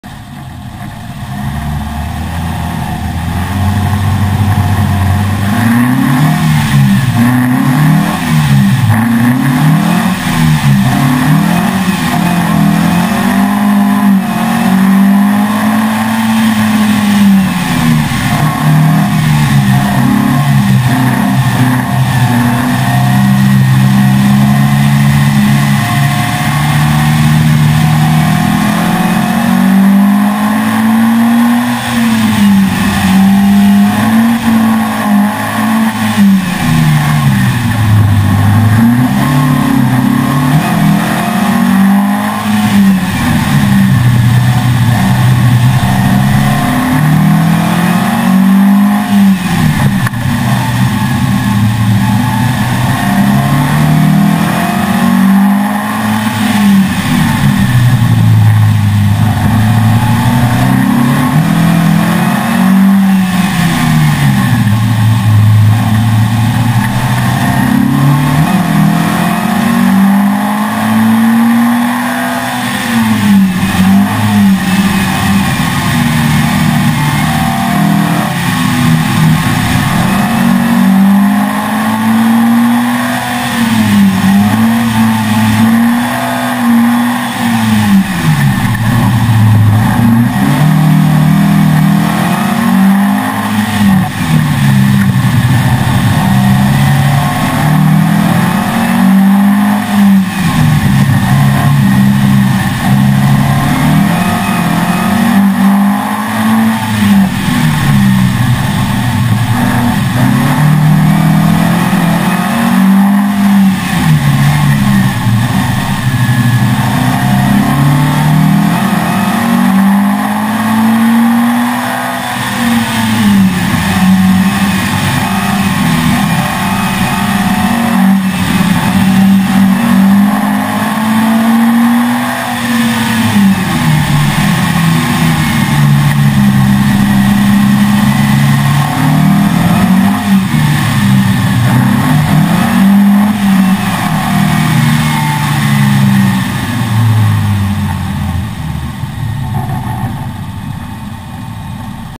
R Sheppard / Broadside Designs Datsun 1200 Coupe - NWCC 2013 Kart Track Lap Dash
Suspension Cam from DAT-110 at nwcc go kart track lap dash